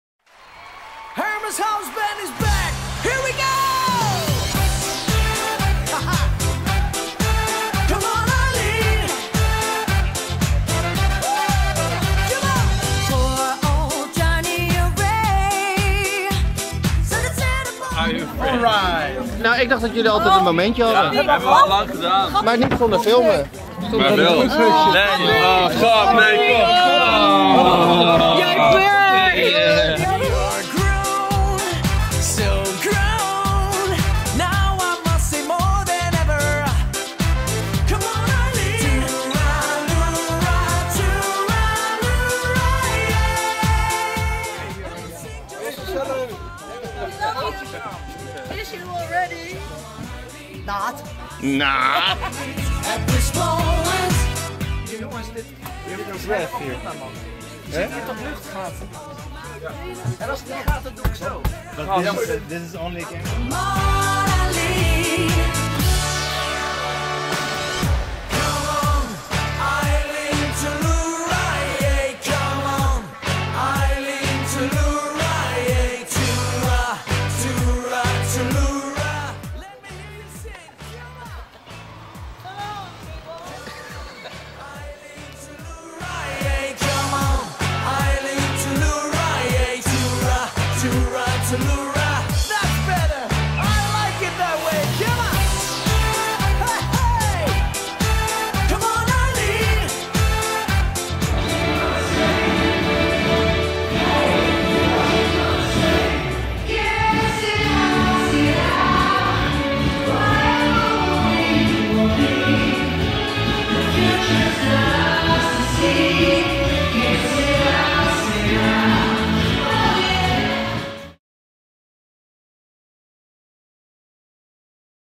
Hermes House Band - Bad Schwartau & Fernsehgarten
17 & 18 augustus 2013 we visited bad Schwartau & after The show we drove into the night...